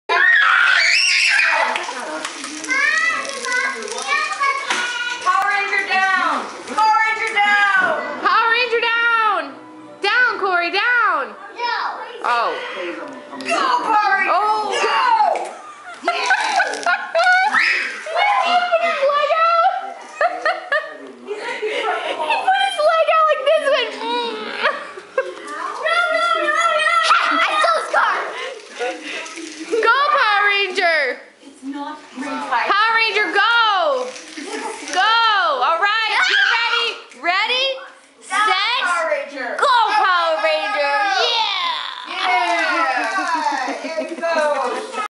Corey pretending to be a power ranger.
Corey chasing his sisters around the house acting like a power ranger.  The funniest part is about 16 secs in when he puts his foot to the side to get ready to CHARGE!